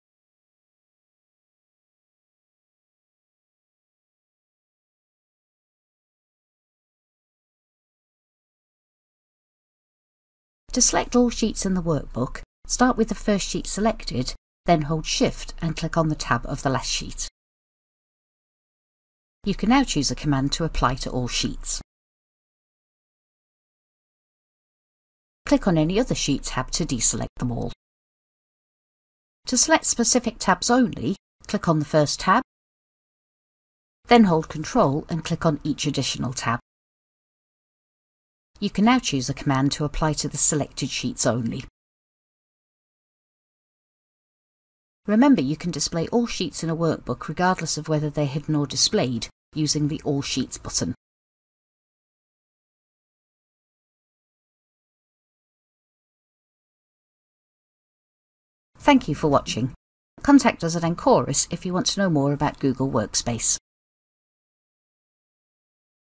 Work on multiple sheets at the same time
You can now select multiple tabs in a Google Sheet and perform actions such as hiding, colour or duplicating the tabs.